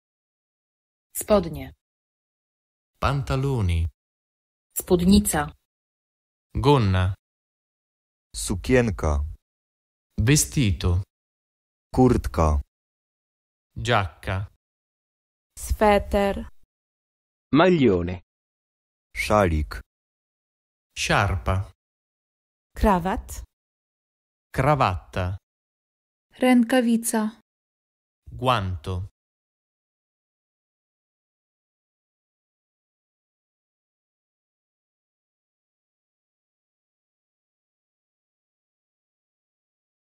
#28 - La lingua polacca - 500 parole. Studiamo la polacco da soli.
500 parole base della lingua polacca.

1  spodnie  pantaloni
2  spódnica  gonna
3  sukienka  vestito
4  kurtka  giacca
5  sweter  maglione
6  szalik  sciarpa
7  krawat  cravatta
8  rękawica  guanto

   Tutte le parole sono sonorizzate da madrelingua.
   Alcune delle parole presentate possono avere i significati supplementari e le sfumature semantiche. Sciegliendo la traduzione di una parola, ci siamo affidati al significato più frequentemente usato.
   Una parte delle parole è stata suonarizzata dagli annunciatori appositamente per il nostro canale. La suonarizzazione di alcune parole è tratta da fonti gratuite su Internet, come ad esempio: